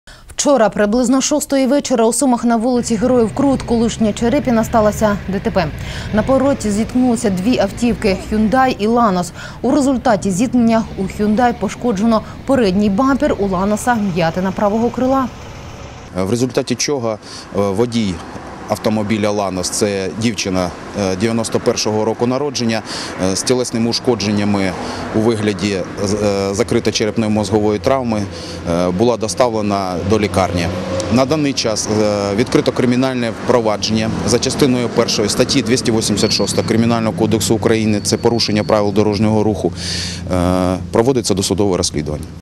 ДТП НА Героїв Крут
Вчора близько шостої вечора у Сумах на вул. Героїв Крут (колишня Черепіна) сталася ДТП.  На повороті зіткнулися дві автівки: Hyundai і Lanos. У результаті зіткнення у Hyundai пошкоджено передній бампер, у Lanos вм’ятина правого крила.